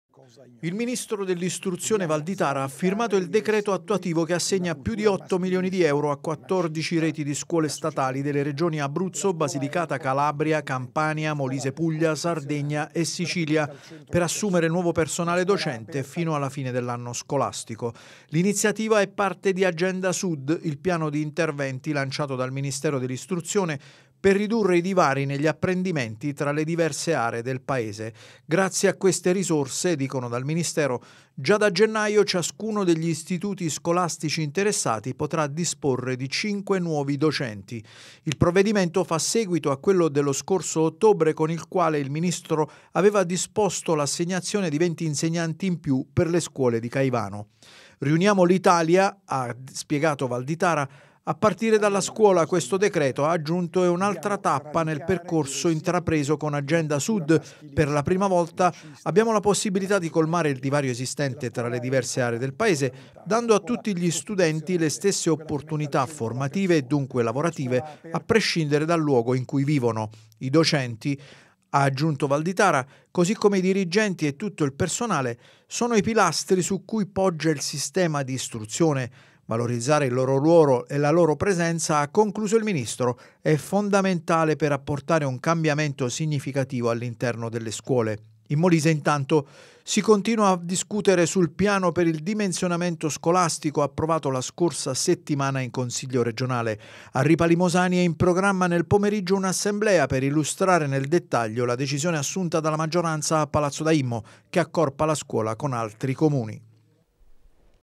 Scuola, Agenda Sud: dal ministero 8 milioni per assumere nuovi docenti
Non si placano le polemiche attorno al mondo della scuola. In Molise, dopo il piano scolastico approvato in consiglio regionale, in programma un'assemblea pubblica a Ripalimosani per discutere come fare fronte all'accorpamento.
Intanto il ministro Valditara ha firmato il decreto con cui saranno possibili nuove assunzioni